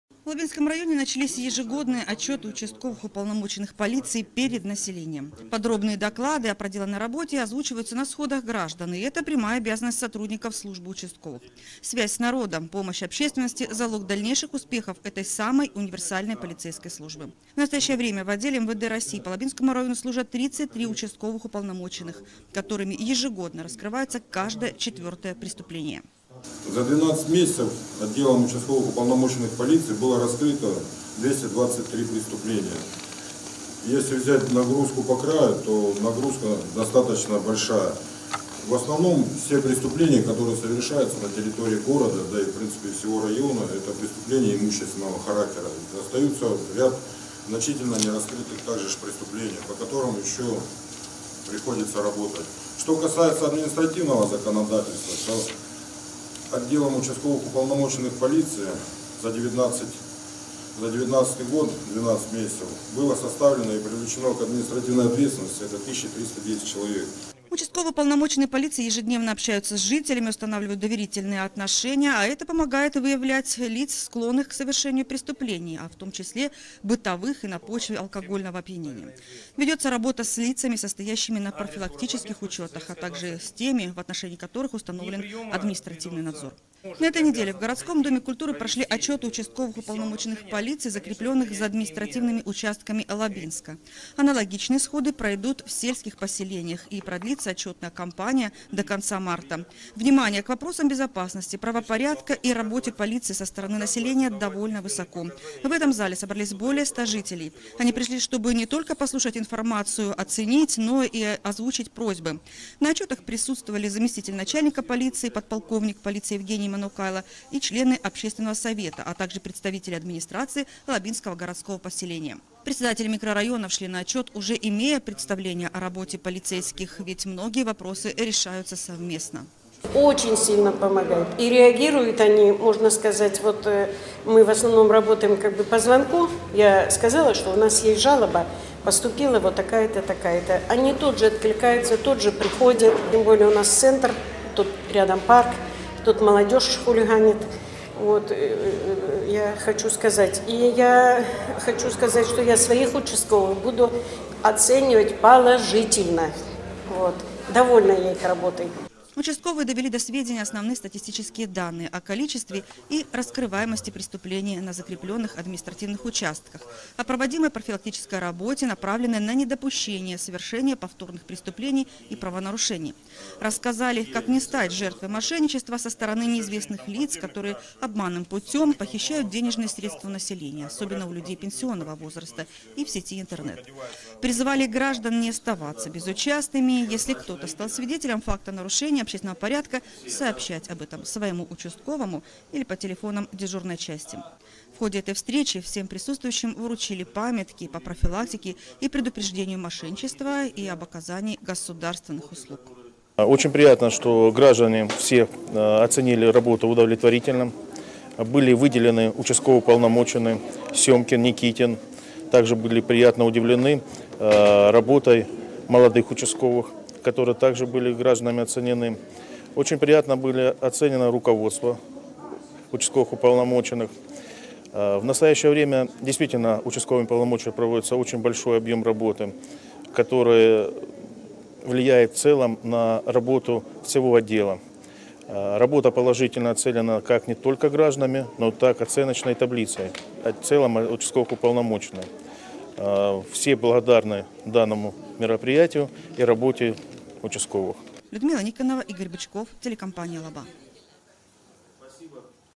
Отчет участковых.
В Лабинском районе проходят отчеты участковых уполномоченных перед населением.  Полицейские отчитываются о своей работе за 2019 год.